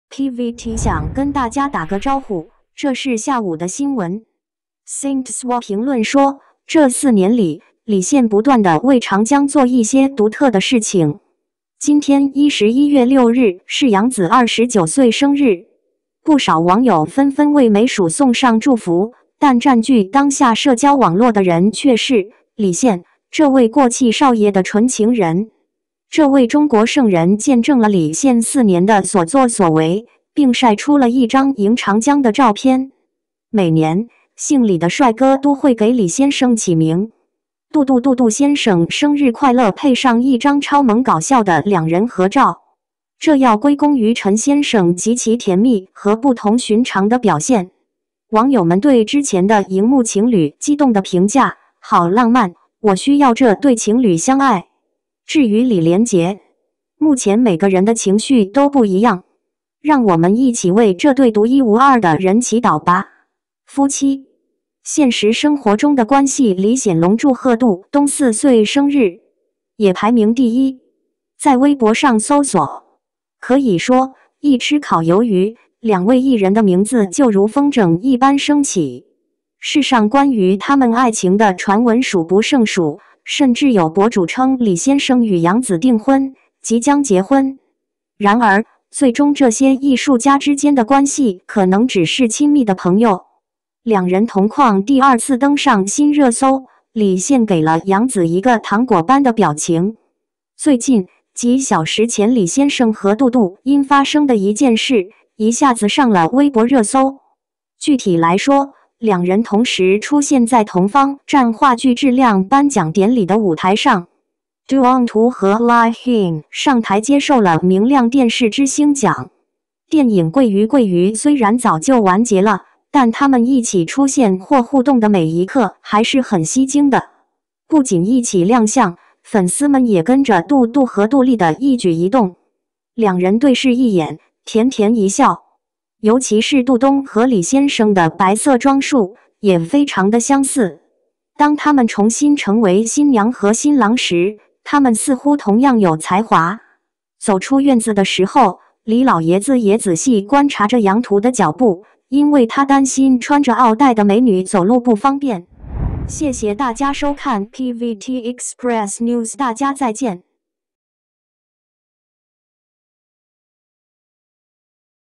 4年来，李现总是为杨紫做一些特别的事情
不仅是李现，就连杨紫都非常深情地表达了自己与“荧屏情人”的关系。
 这4年李现一直都是这样做的，就是晒出一张给杨紫拜年的照片。 每年，李姓帅哥都会喊杨紫老师：“祝杨紫老师生日快乐”，并附上两人的萌萌哒搞笑照片。